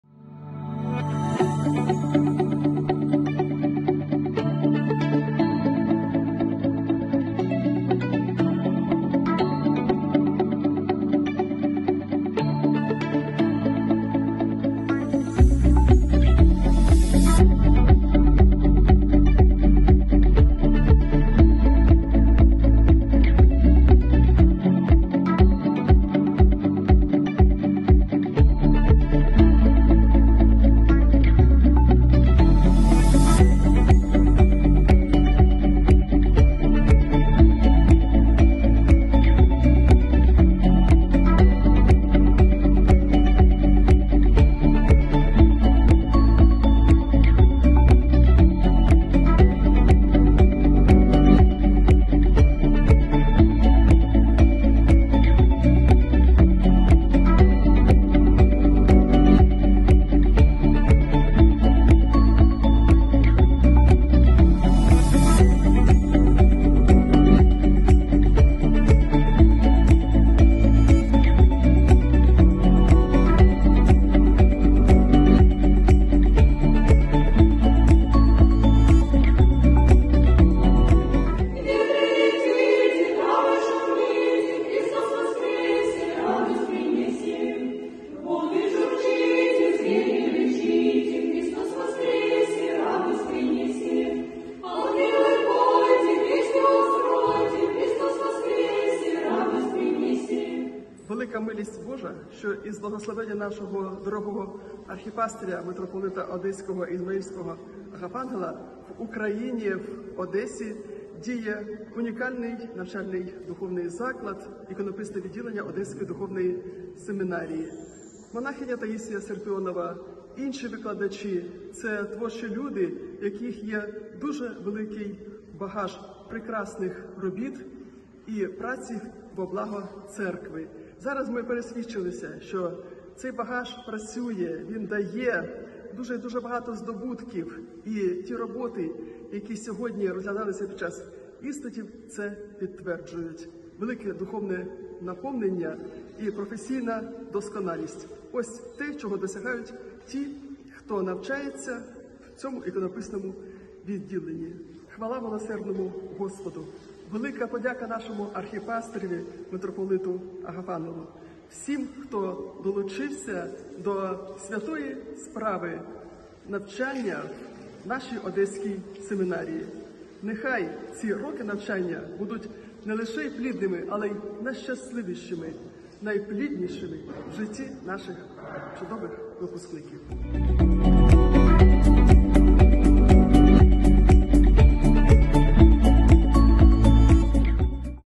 10 июня 2024 года в регентском отделении Одесской духовной семинарии прошла защита дипломных работ.
🗓️10 червня 2024 року у регентському відділенні Одеської духовної семінарії пройшов захист дипломних робіт.

👆Кожна випускниця підготувала унікальний дипломний проєкт, у якому відобразила глибину своїх теоретичних знань та практичних умінь.

🙏Наприкінці заходу ректор семінарії протоієрей Дмитро Яковенко звернувся до вихованок та вихованців з напутнім словом, у якому побажав зустріти нову сторінку їхнього життя з усіма труднощами та випробуваннями з терпінням та благоговійним серцем, щоб незабутній час, проведений у духовній школі, став для них цікавим, корисним та душерятівним.